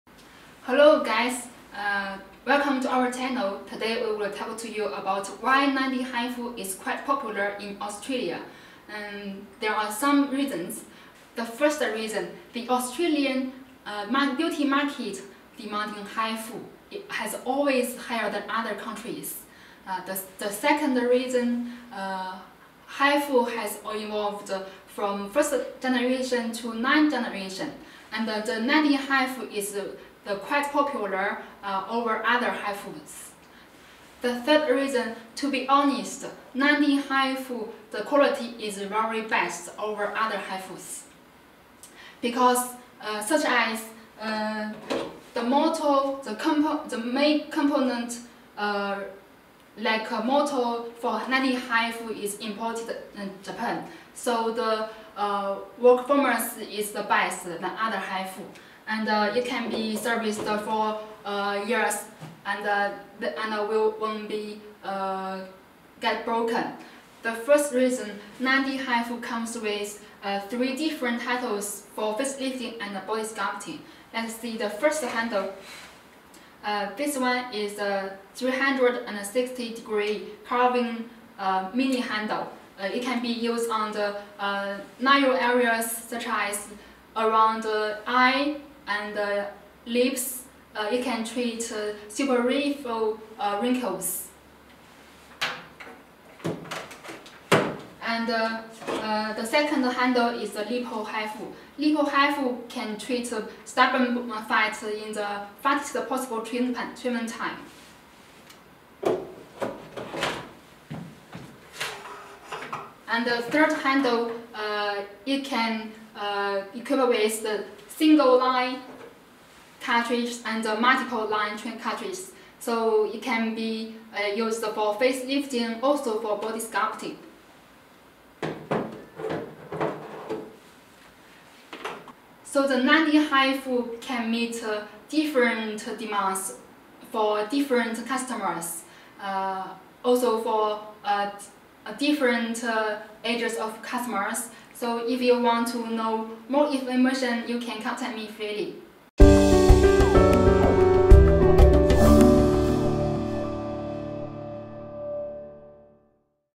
HCBEAUTY 9d hifu machine australia FU4
Popular in australia 9d hifu machine

Features:
1) For face: face lifting, wrinkle removal, skin tightening, fine wrinkle removal, neck wrinkle removal, double chine removal.

2) For body: abdomen fat reduce, arms fat reduce, legs fat reduce, thighs fat reduce, butt lifting, fat reduce, slimming